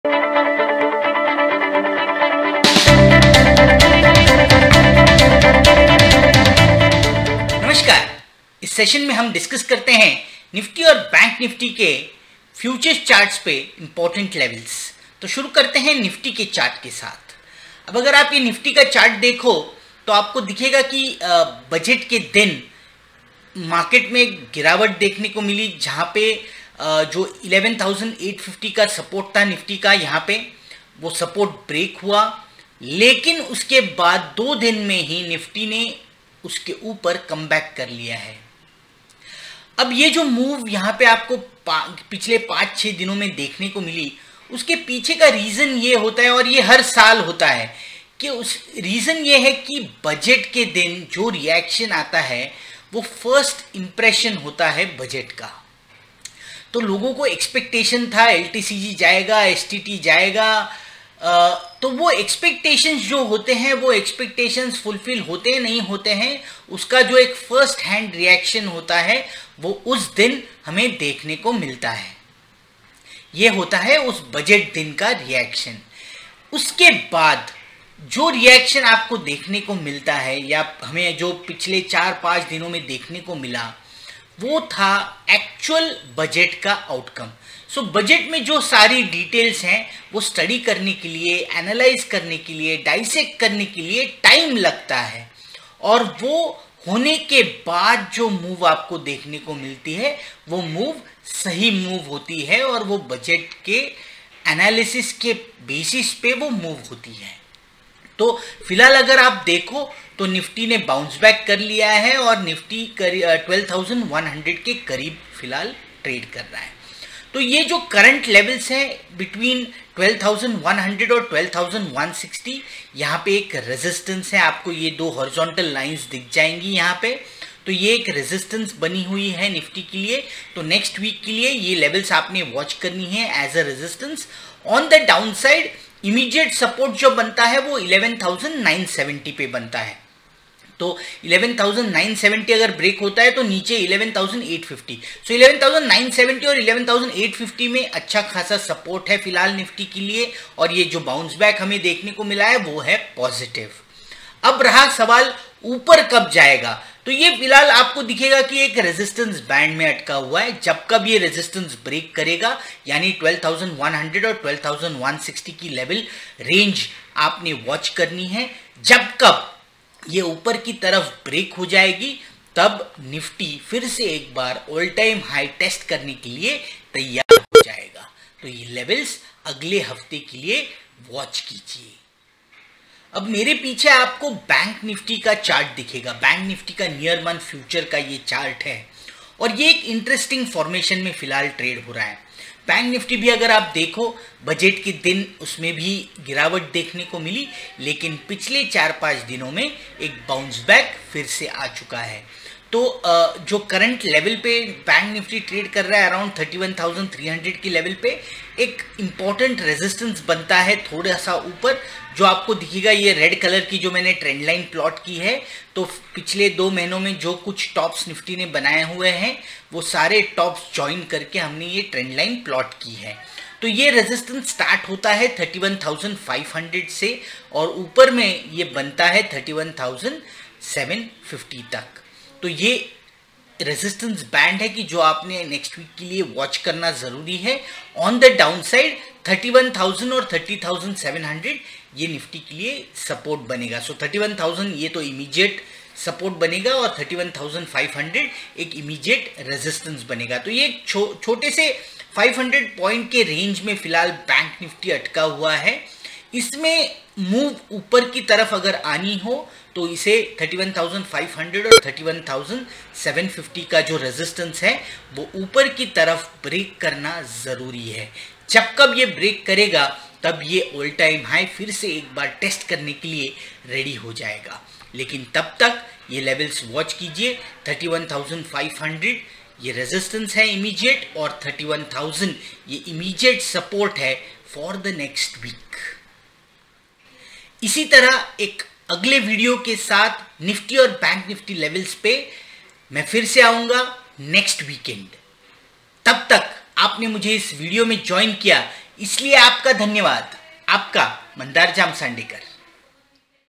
Nifty & BankNifty Future (8 FEB 2020) Chart View by Mandar Jamsandekar
WEEKLY UPDATE - NIFTY & BANKNIFTY FUTURES CHART for 10 to 14 FEBRUARY 2020.

LEARN STOCK MARKETS, TECHNICAL ANALYSIS, GET YOUR STOCK RELATED QUERIES SOLVED!

Making Technical Analysis Simple For You!

In this video i discuss the technical view on the NIFTY and BANKNIFTY Near Month Future Chart. This view is applicable for the Trading Week between 10 to 14 FEBRUARY 2020. I am sure this input will be helpful to traders as well as investors to take Good INFORMED Decisions.